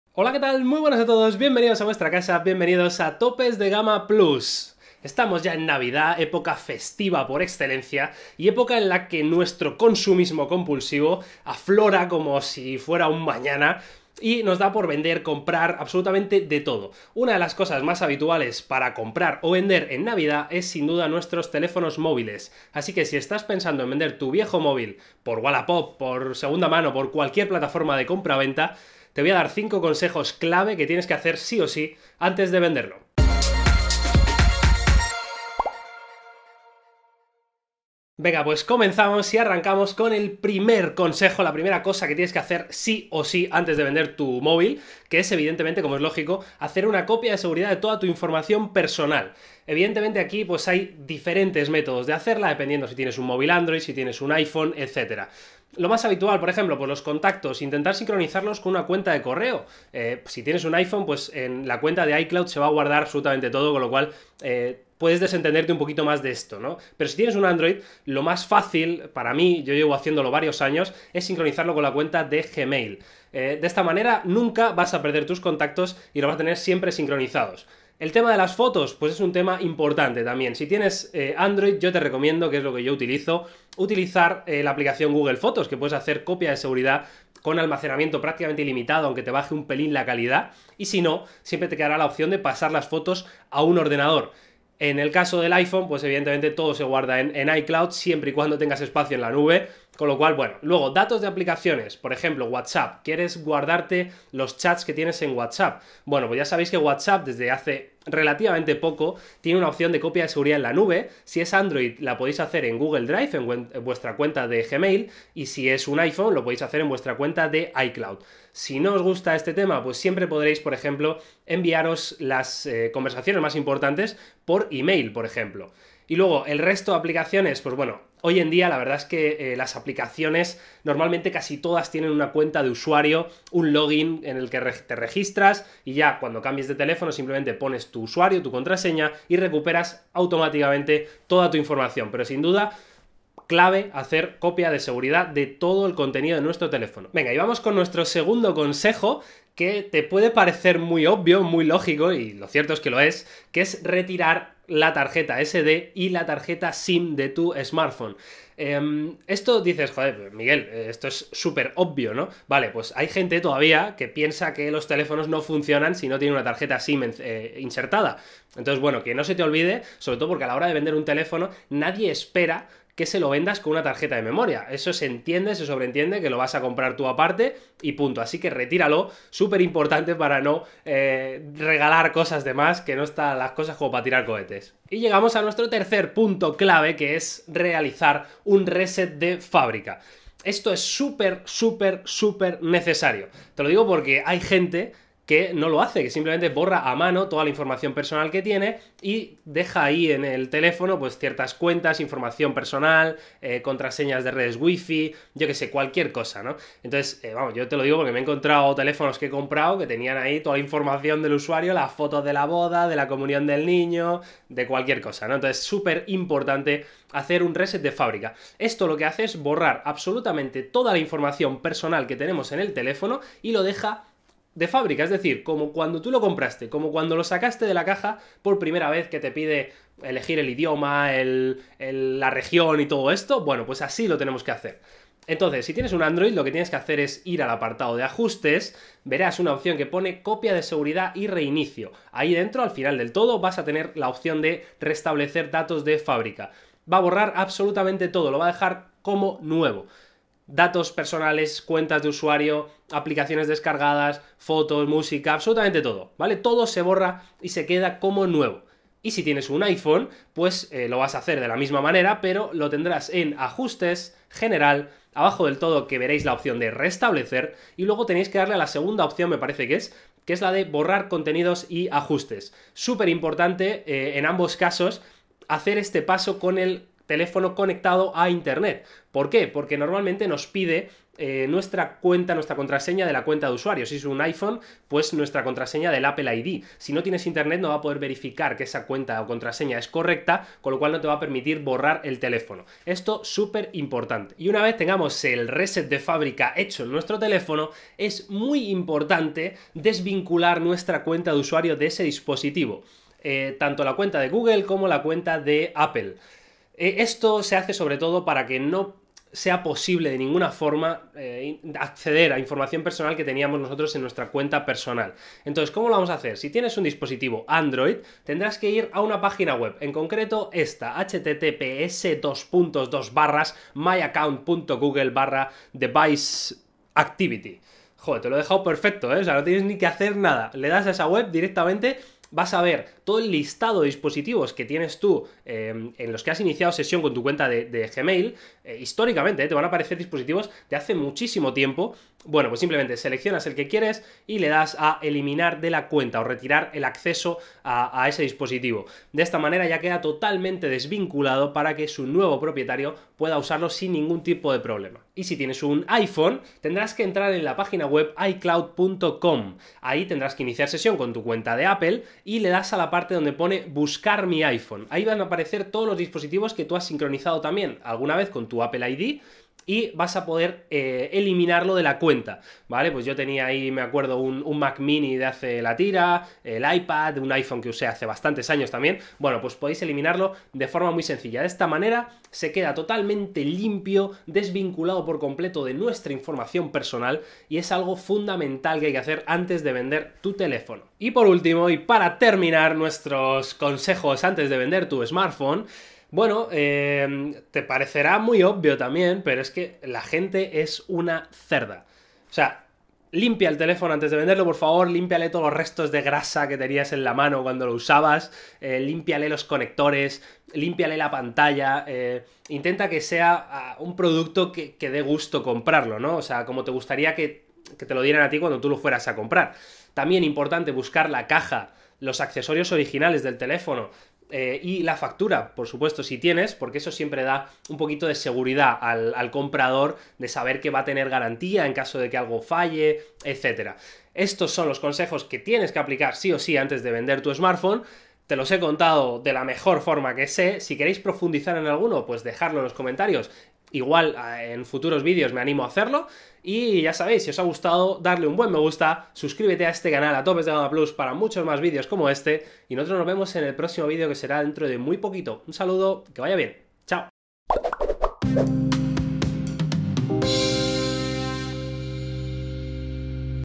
5 Cosas que DEBES HACER antes de VENDER TU MÓVIL por Wallapop!!
►► ¿Estás pensando en vender tu móvil? Pues echa el freno. Antes de venderlo repasa estas 5 cosas clave que tienes que hacer para evitar riesgos.